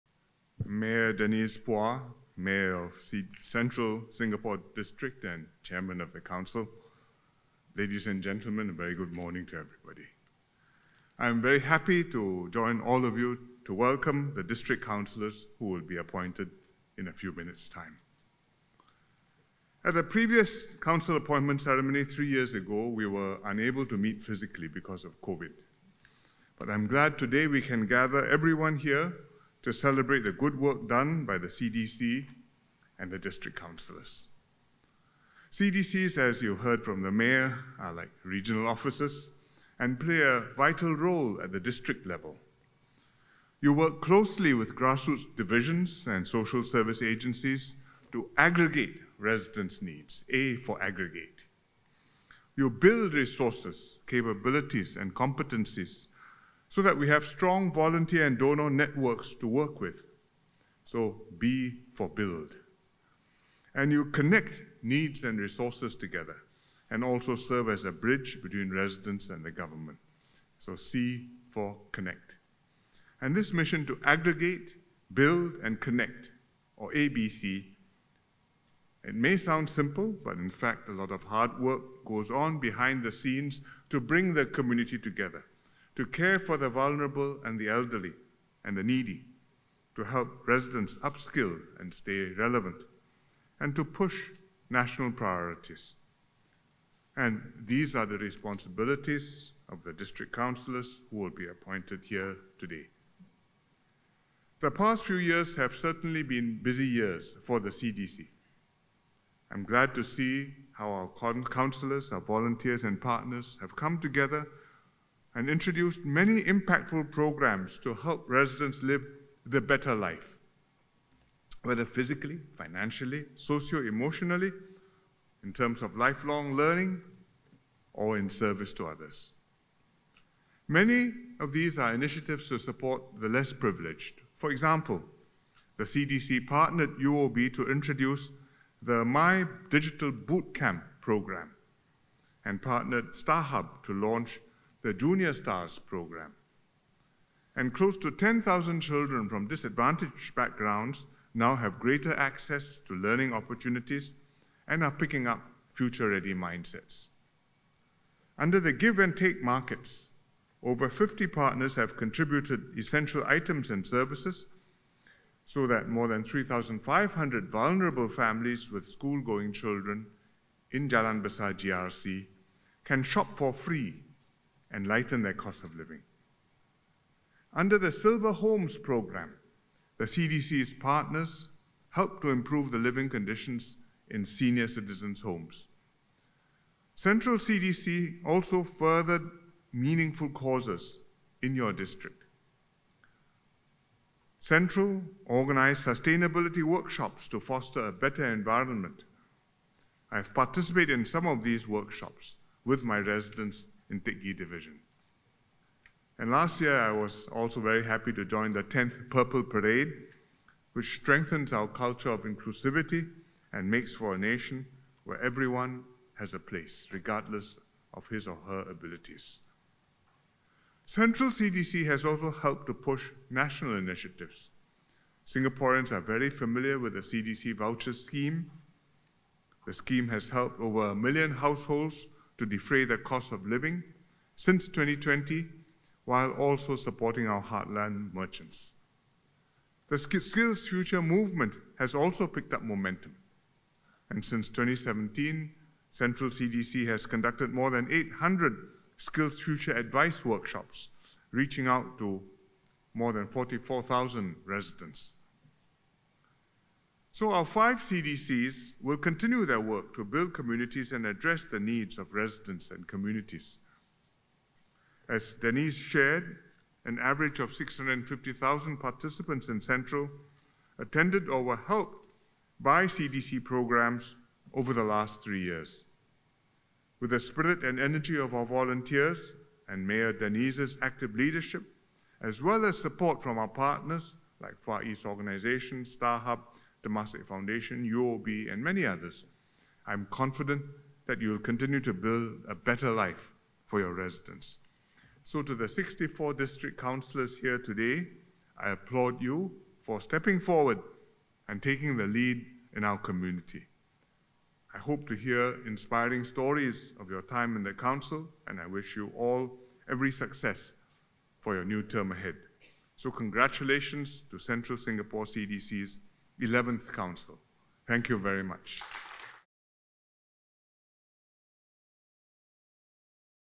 PM Lee Hsien Loong at the Central Singapore CDC 11th Council Appointment Ceremony
Prime Minister Lee Hsien Loong spoke at the Central Singapore Community Development Council 11th Council Appointment Ceremony on 25 September 2023.

(PMO Video by Bruce D'cruz)